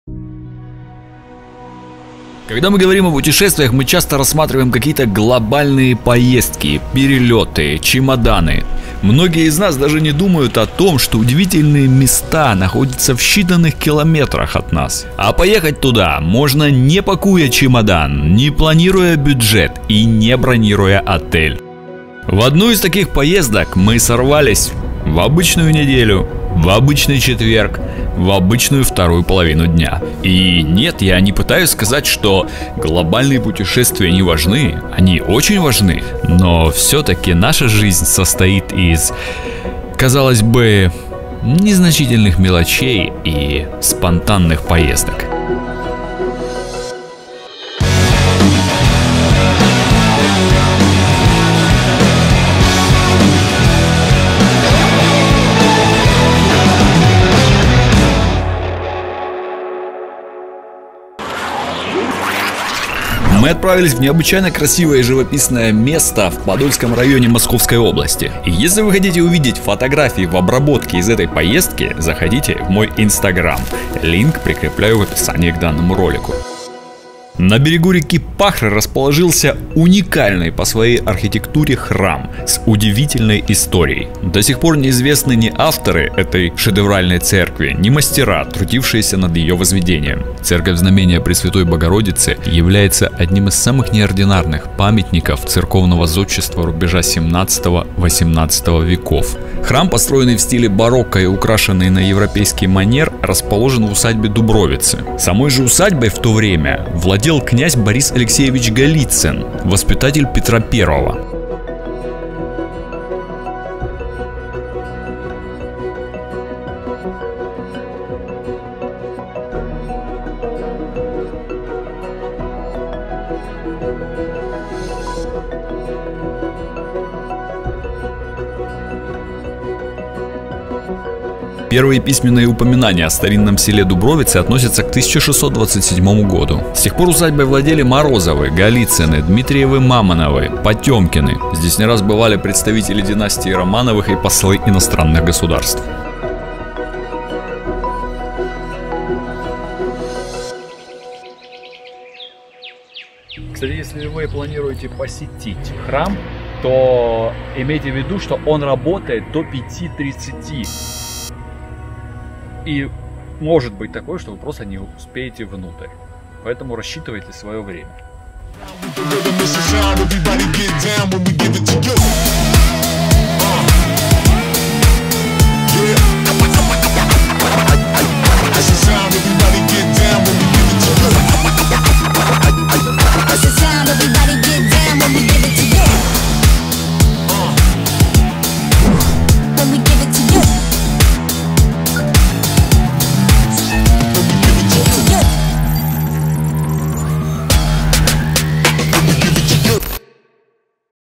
Уникальные памятники Подмосковья. Усадьба Дубровицы и Храм Знамения в Подольске
Друзья, хочу, чтобы вы сегодня посмотрели на уникальные памятники Подмосковья —  Усадьба Дубровицы и Храм Знамения в Подольске. Мы спонтанно, посреди недели решили съездить в небольшое путешествие, и я рад поделиться с вами своими впечатлениями.

Церковь знамения Пресвятой Богородицы — это самый неординарный памятник церковного зодчества, рубежа 17-18 вв.

Храм построен в стиле барокко и украшен на европейский манер. Находится в усадьбе Дубровицы, в городе Подольск, в нескольких часах от Москвы.

Ранее, эта усадьба принадлежала князю Борису Алексеевичу Голицыну, воспитателю Петра I. Сейчас же — это знаменитая достопримечательность Подмосковья, куда съезжаются туристы, чтобы оценить уникальное архитектурное строение и сделать крутые фото.